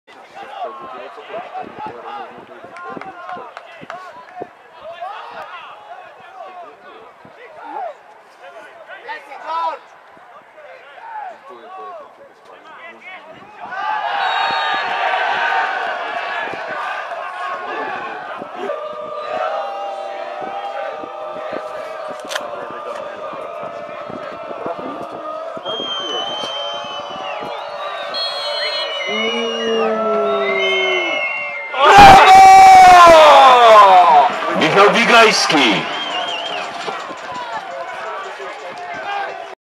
mksswit.pl MKS Świt Nowy Dwór Maz. - Unia Tarnów obroniony rzut karny Węgrzyna
mksswit.pl MKS Świt Nowy Dwór Maz. - Unia Tarnów obroniony rzut karny Węgrzyna prze Michała Bigajskiego